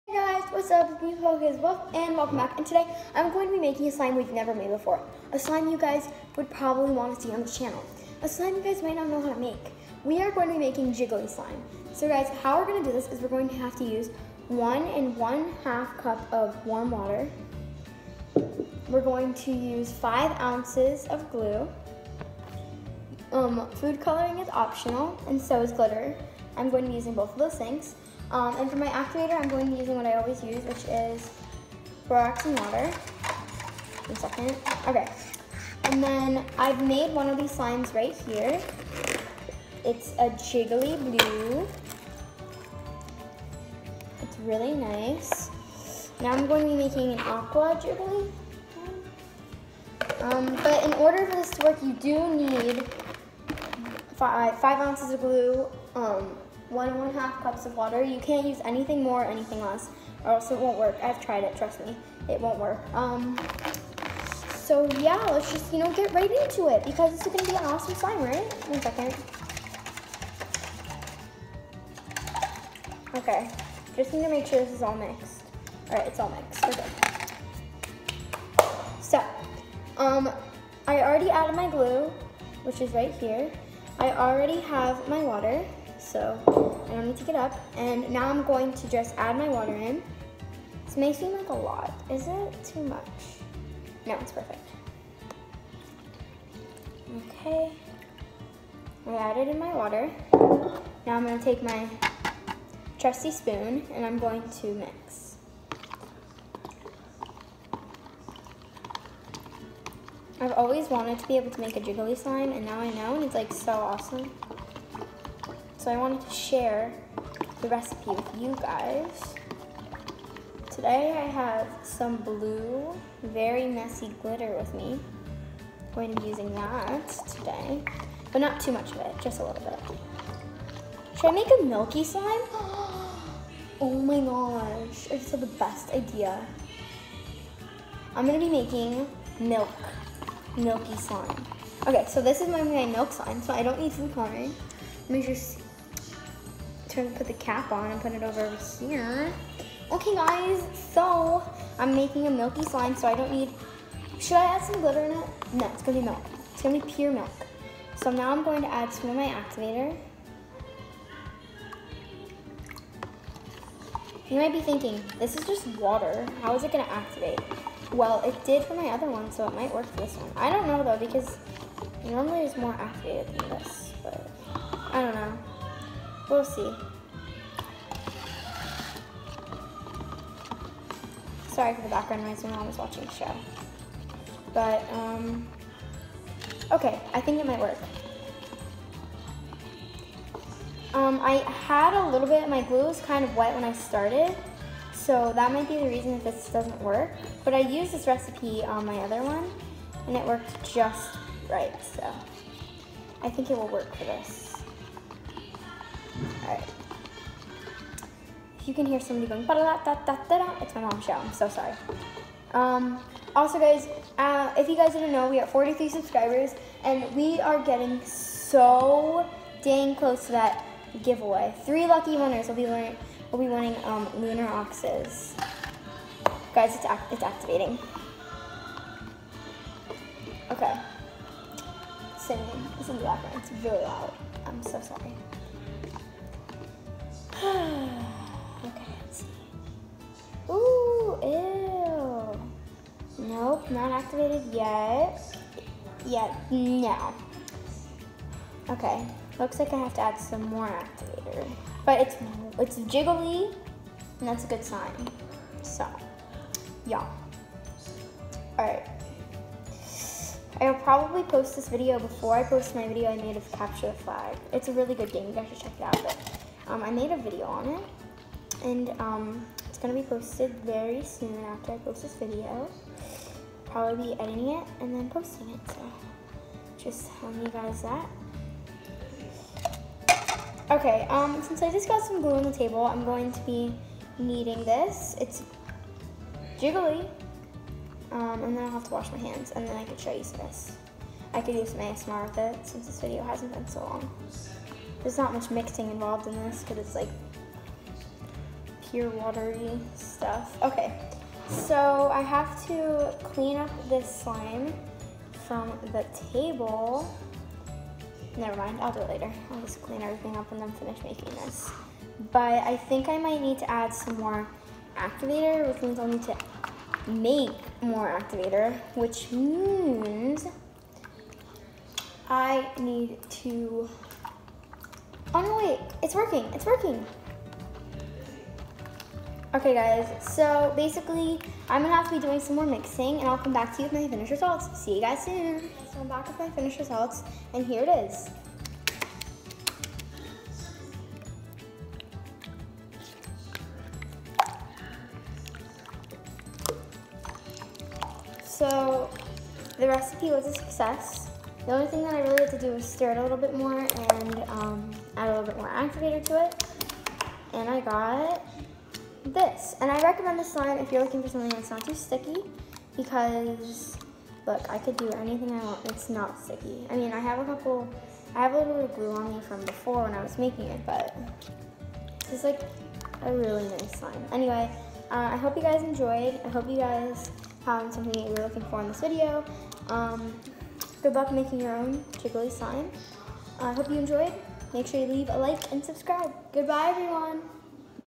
Making jiggly slime!!!
Ingredients:
5 oz of glue
1 1/2 cup of water
Borax and water to make activator